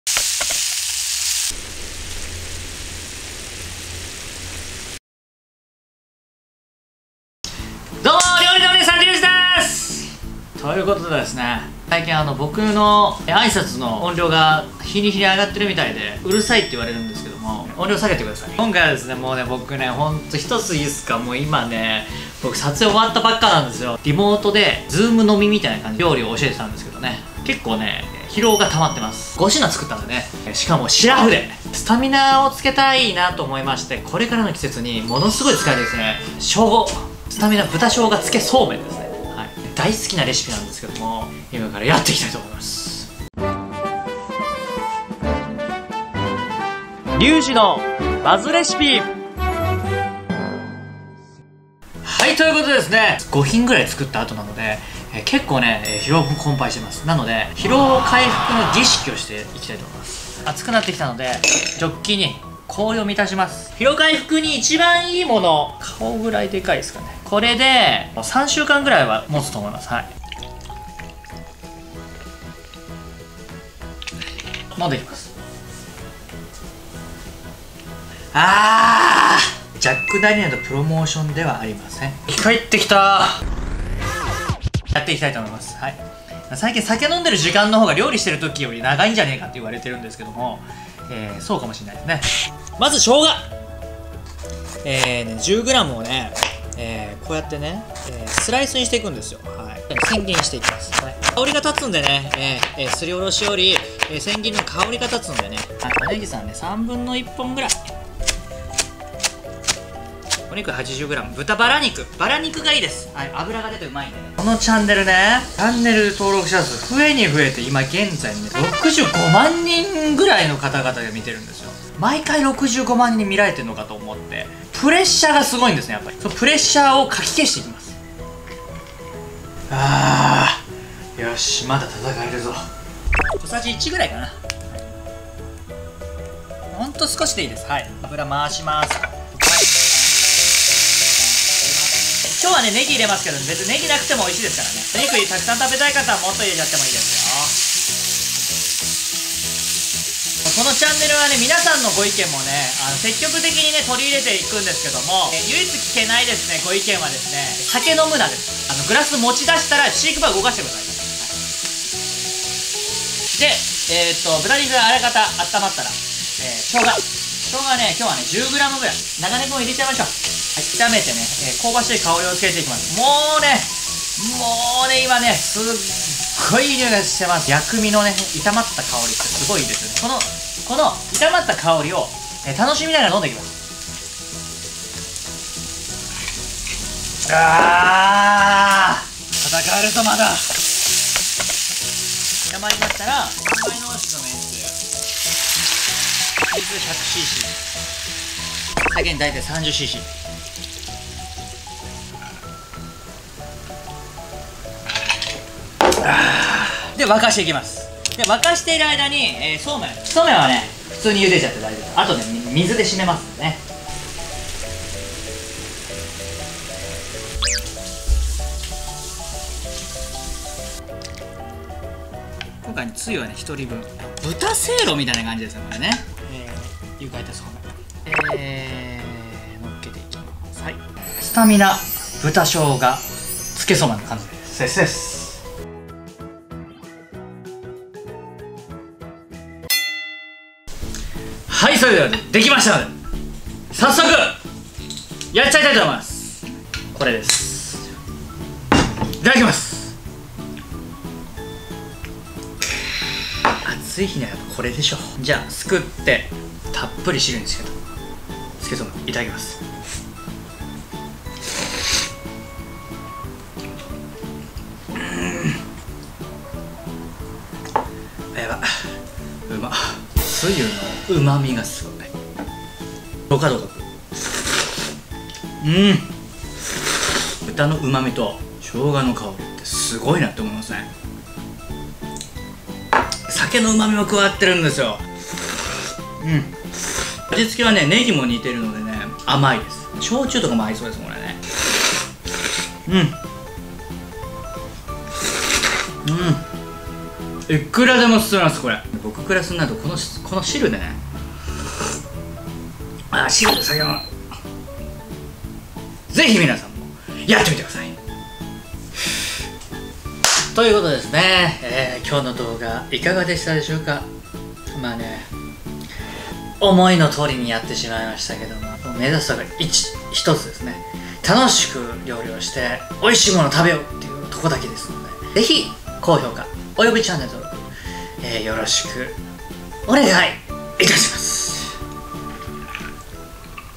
食欲不振をぶっとばす！史上最強の豚ダレで食べる【スタミナ豚そうめん】
【スタミナ豚そうめん】

★今回のレシピはこちら↓
・そうめん　　１００ｇ
・生姜　　　　１０ｇ
・めんつゆ　　５０㏄
（３倍濃縮のものを使ってます）
・水　　　　　１００㏄
・酒　　　　　３０㏄
・小ネギ　　　適量
★お好みで　ラー油

フリーBGM
DOVA-SYNDROME